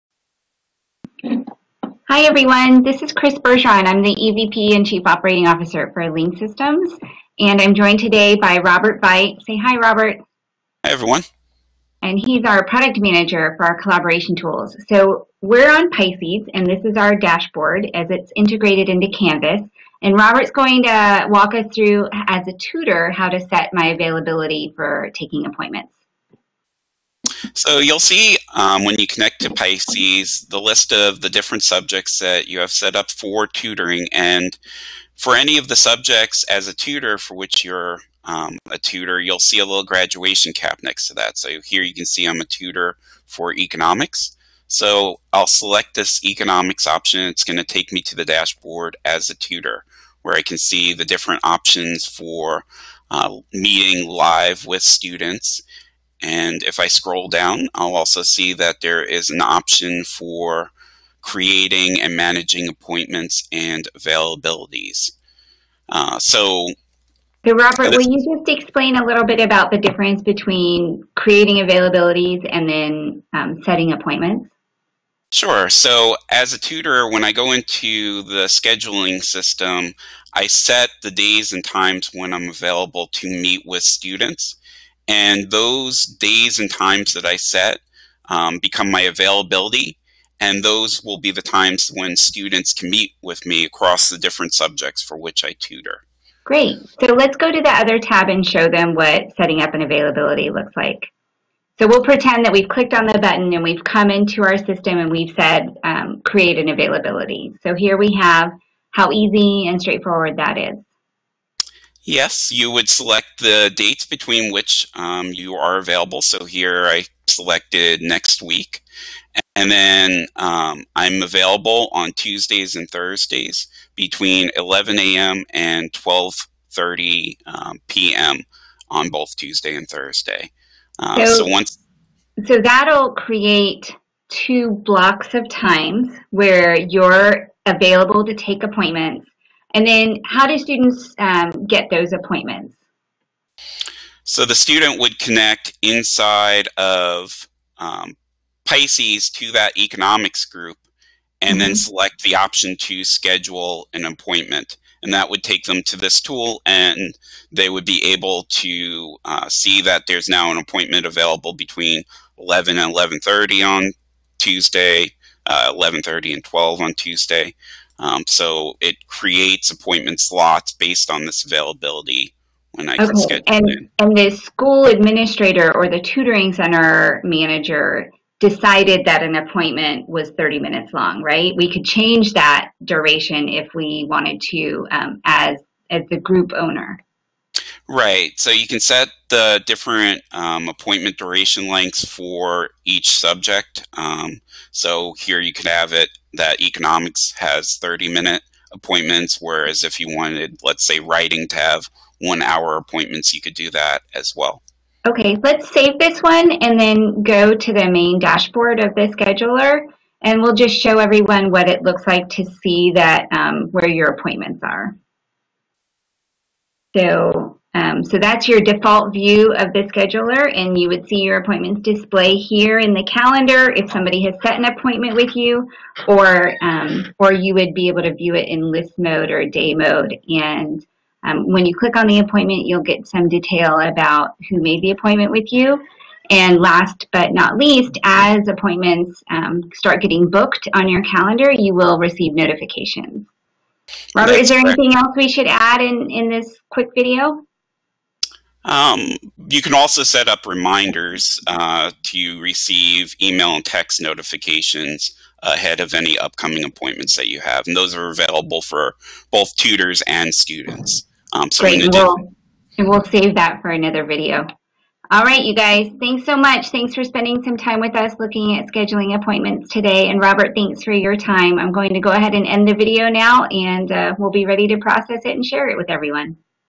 Creating Tutor Availability for Appointments in Pisces
In this video, we show you how to set your tutor availability for appointments.